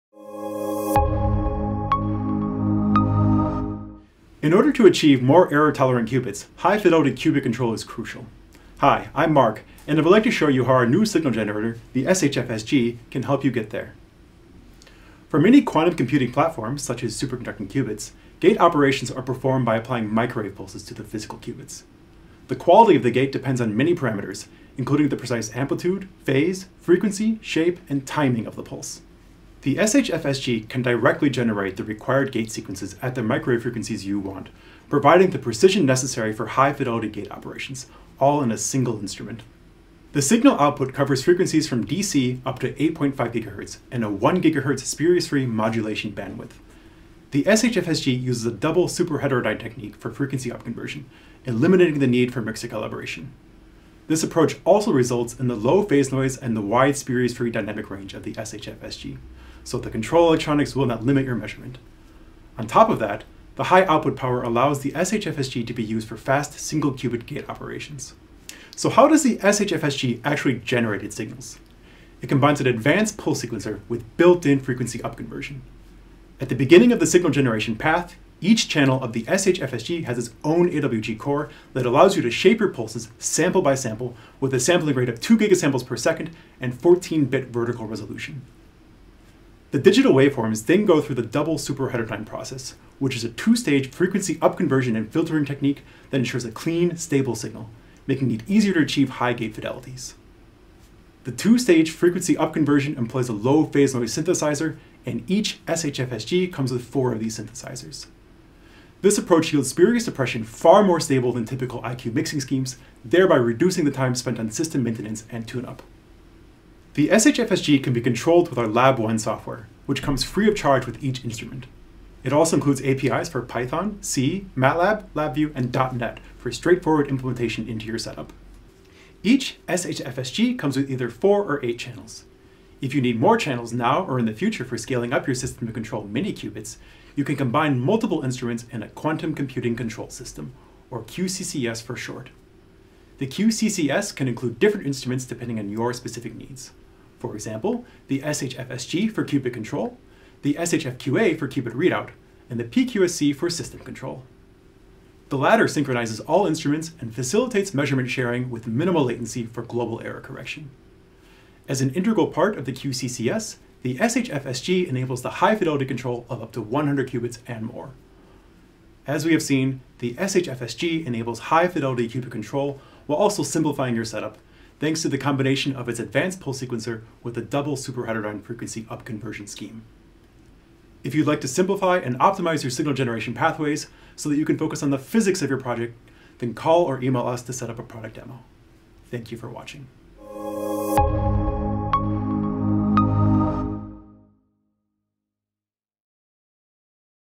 SHFSG 8.5 GHz Signal Generator
Watch how the Zurich Instruments SHFSG Signal Generator can help you perform high-fidelity qubit control without mixer calibration.
The SHFSG Signal Generator outputs signals from DC up to 8.5 GHz without the need for mixer calibration, and provides spurious-free, low-noise signals in a 1 GHz bandwidth. Specially designed for superconducting and spin qubits, the signal generator takes advantage of the double super-heterodyne frequency conversion technique to ensure better linearity and fewer spurious tones than with standard IQ-mixer-based methods. The SHFSG Signal Generator can be programmed to generate complex gate sequences. As a result, the SHFSG Signal Generator generates clean and stable pulses without requiring users to spend time on mixer calibration or system maintenance.
The SHFSG Signal Generator integrates seamlessly with all devices in the Zurich Instruments Quantum Computing Control System and is available in three variants: 2-channel, 4-channel, and  8-channel.